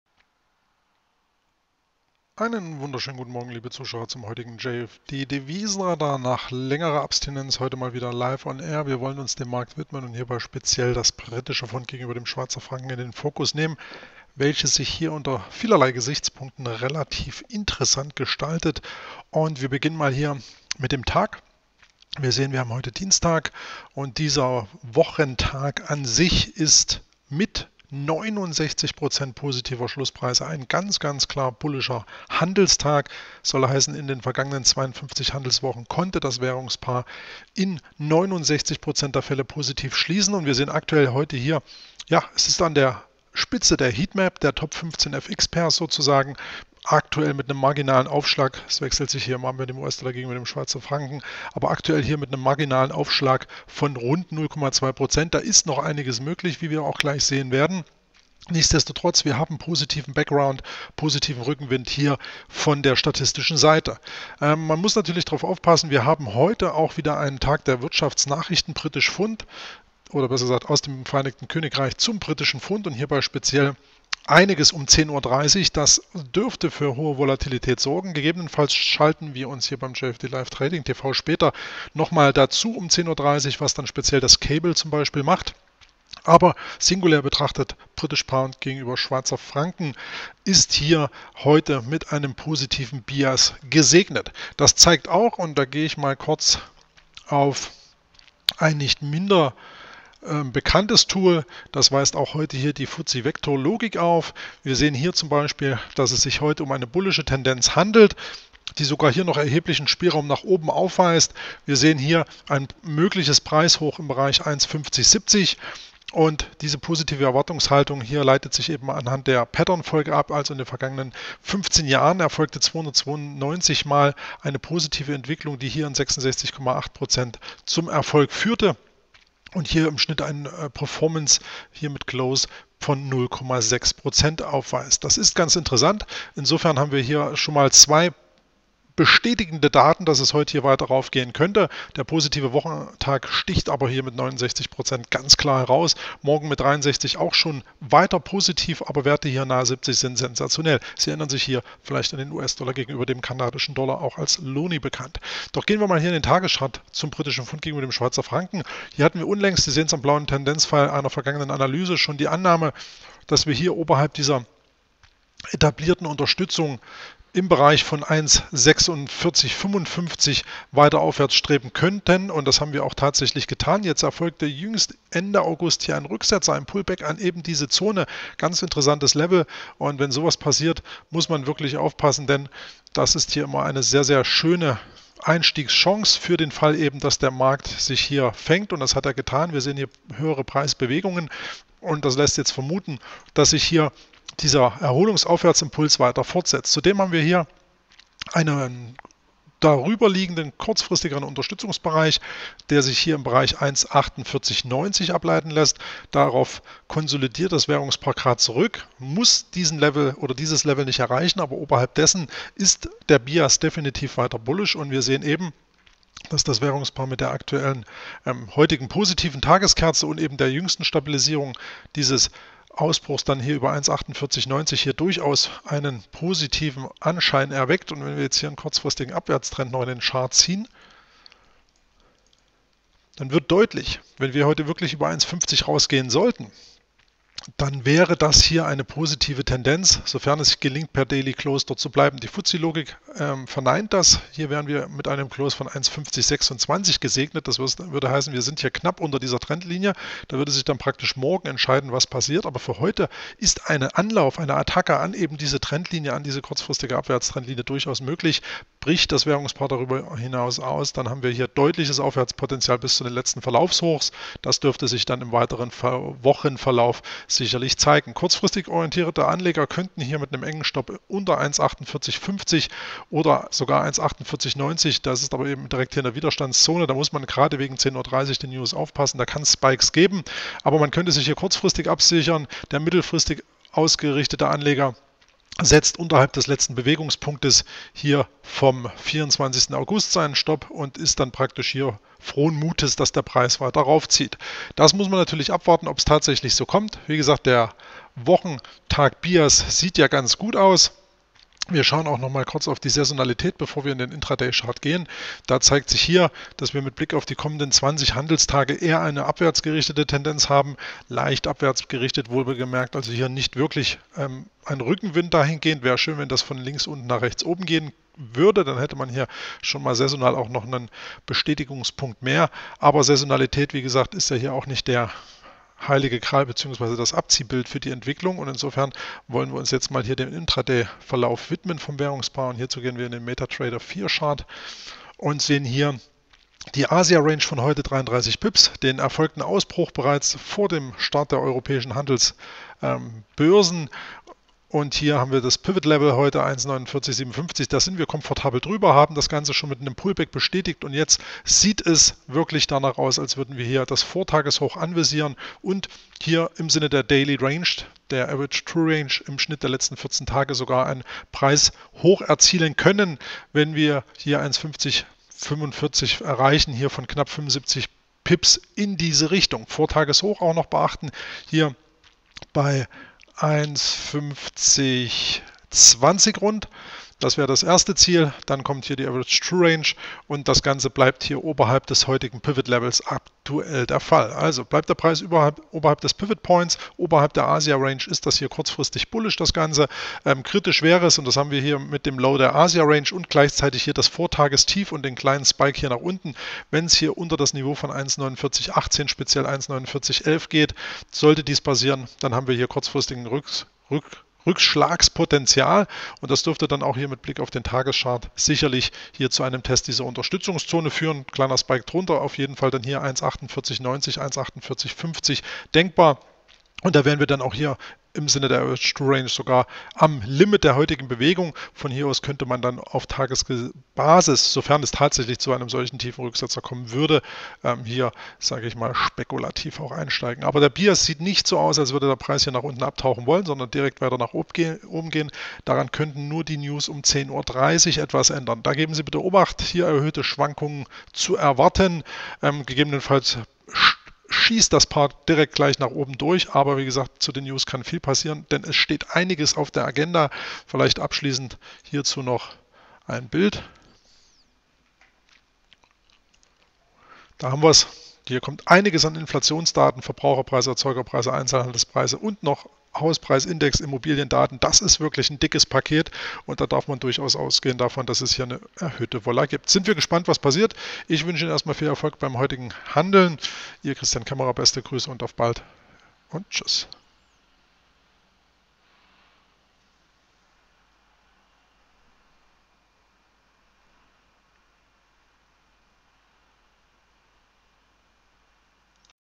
JFD Devisenradar: Beim GBP/CHF verspricht sich noch mehr als bisher
Das GBP/CHF weist seit Ende August eine Stabilisierungstendenz auf, doch wie kann es Intraday und auch auf Tagesbasis weitergehen? 69% positive Schlusspreise sind jedenfalls schon höchst bullisch zu werten.